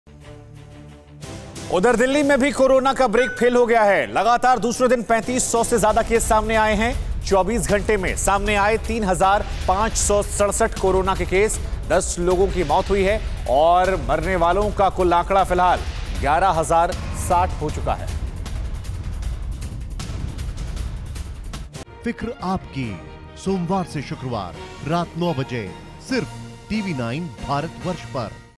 Delhi में Corona का ब्रेक फेल, बीते 24 घंटे में आए 3567 नए केस
दिल्ली में कोरोना के चलते हालात बेहद खराब हो रहे हैं.बीते 24 घंटे में कोरोना के 3567 नए मामले सामने आए हैं.